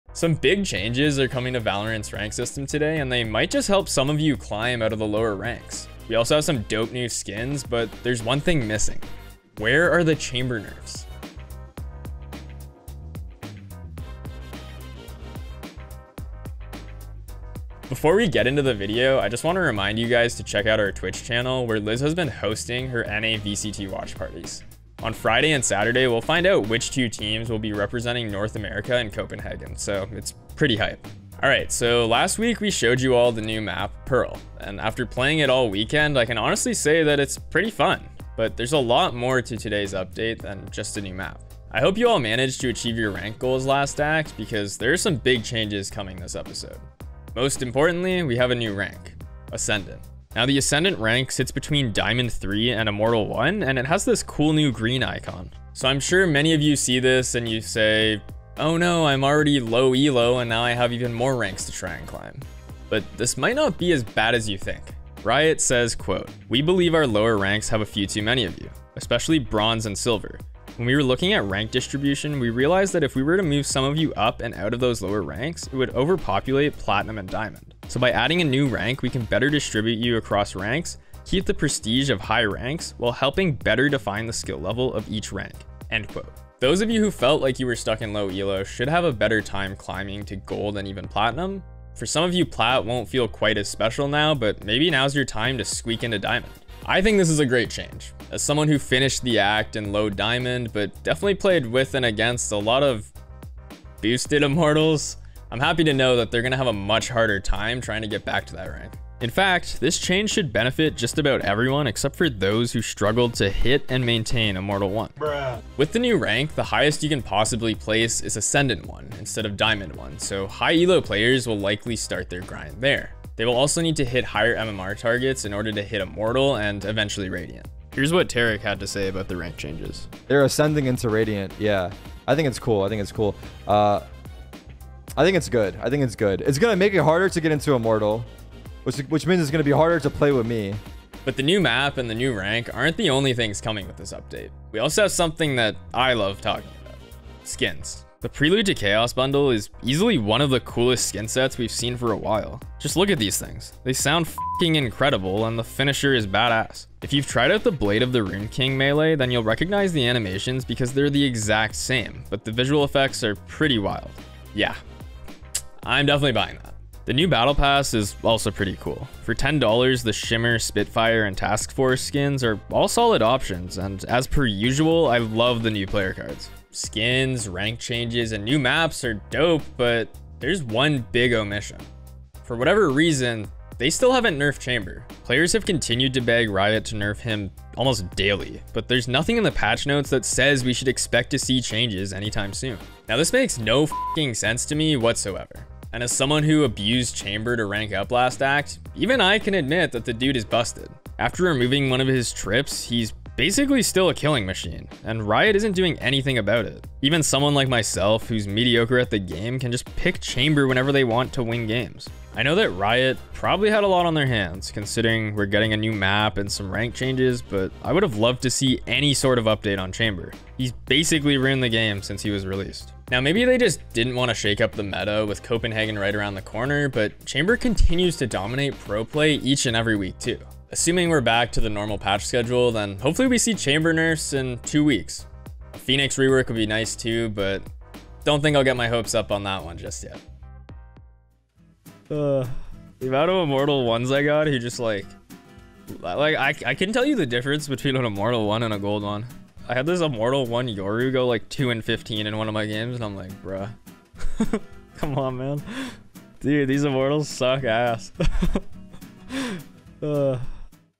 Where The F@*k Are The Chamber Nerfs!?
Some big changes are coming to VALORANT’s ranked system today and they might just help some of you finally climb your way out of the lower ranks.

We've got a brand new rank: Ascendant.

We also have some dope new skins but there’s one thing missing – where are the Chamber nerfs?

Written & Hosted by: Niall McCrossan (@niallmccrossan)
Edited by: Brendan Fahey (@Drillbit_)
Thumbnail by: Bennett Grainger
Produced by: Keith Capstick (@KeithCapstick)